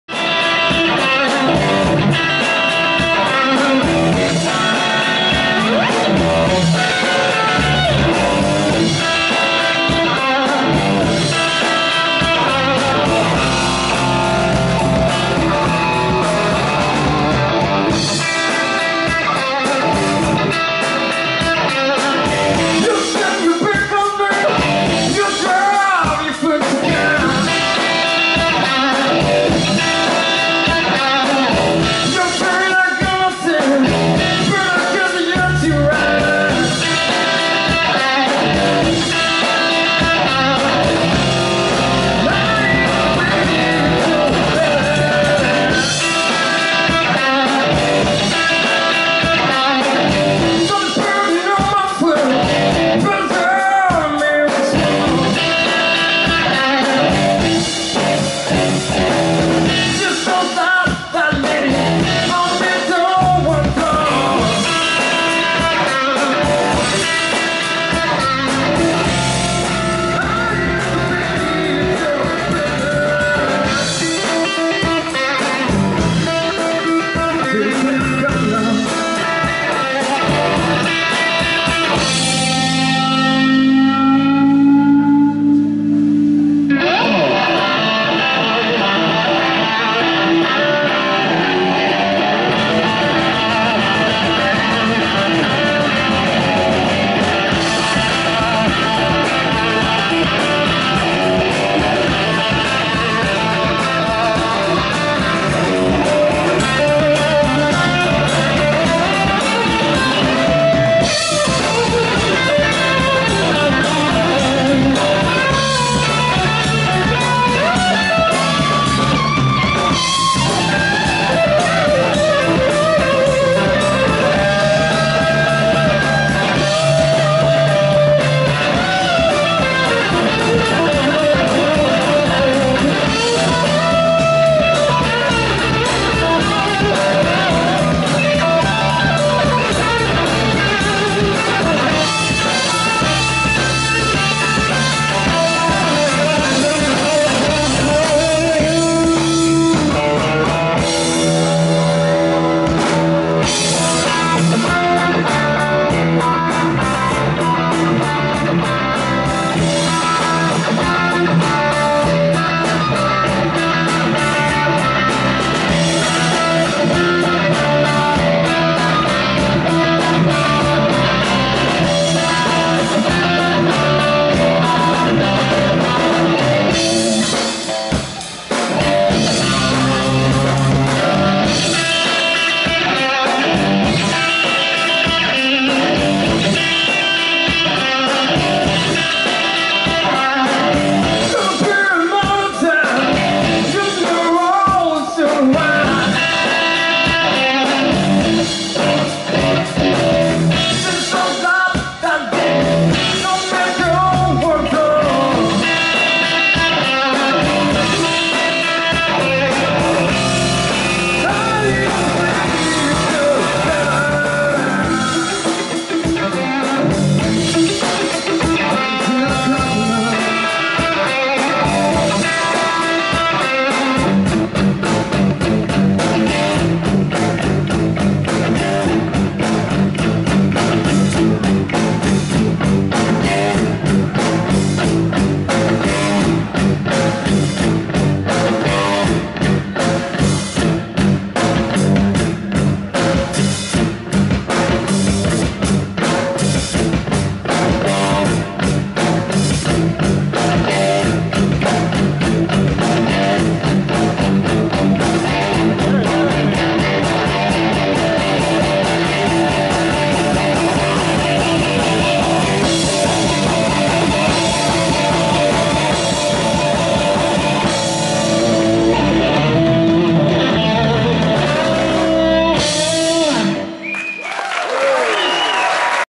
BLUES FOX en LIVE @ " L'Escale de Morteau le 22/04/2022
Concert de blues fox à L'Escale de Morteau le 22/04/2022 ...! 🎸🎸🎸